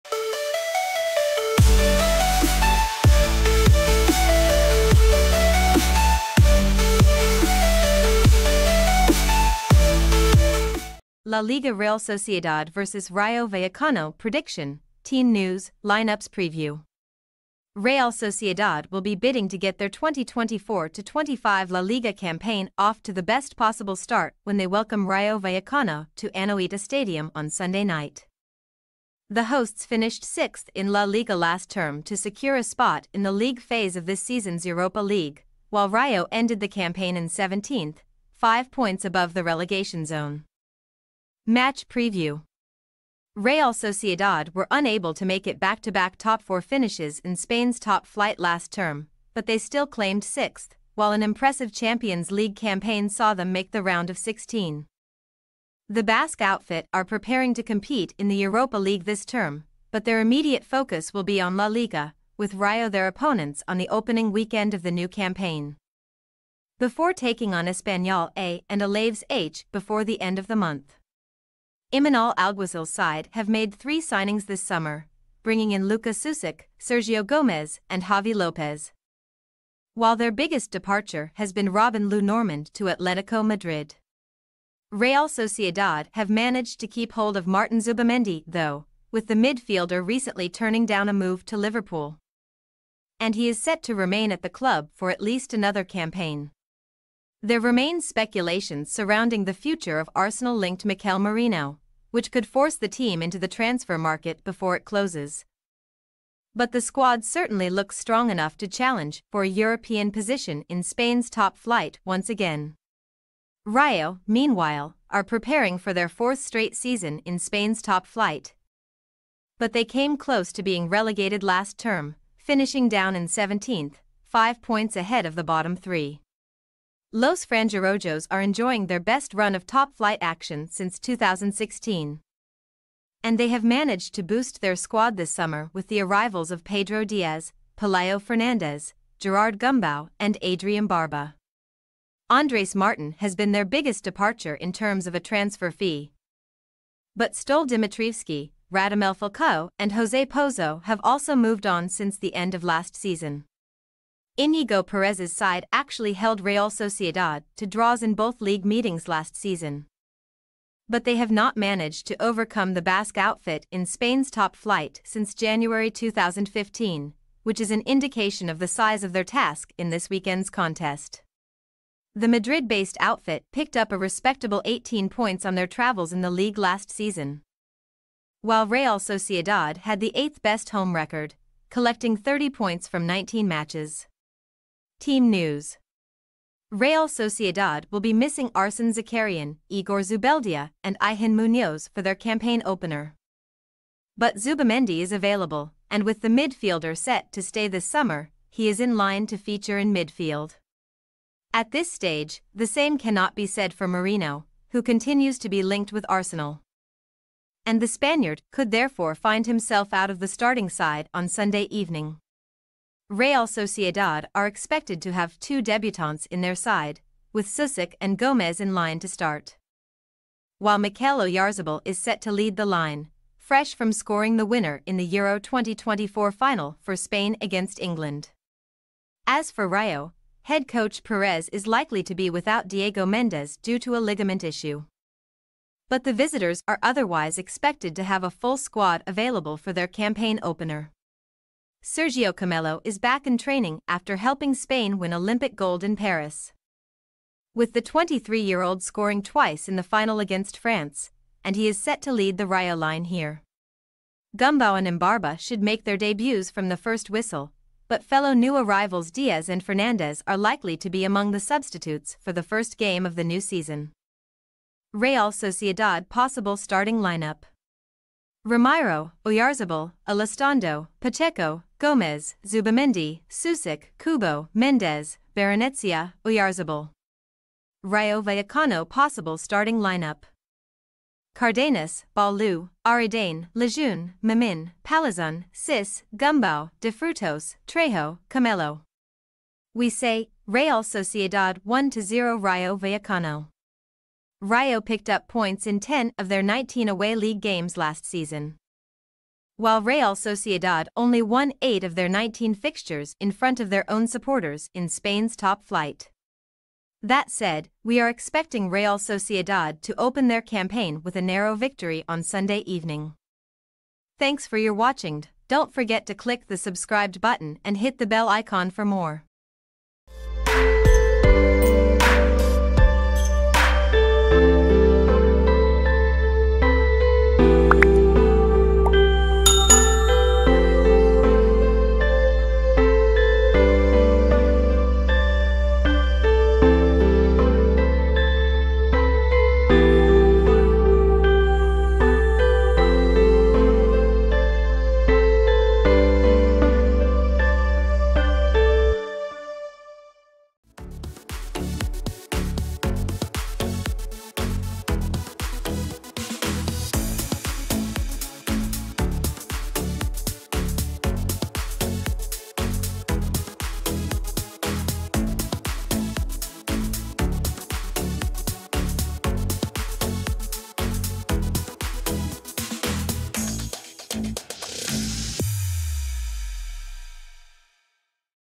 La Liga | Real Sociedad vs. Rayo Vallecano - prediction, team news, lineups | Preview
We say: Real Sociedad 1-0 Rayo Vallecano
Rayo picked up points in 10 of their 19 away league games last season, while Real Sociedad only won eight of their 19 fixtures in front of their own supporters in Spain's top flight. That said, we are expecting Real Sociedad to open their campaign with a narrow victory on Sunday evening.

00:00 Introduction
00:11 Contents
05:36 Outro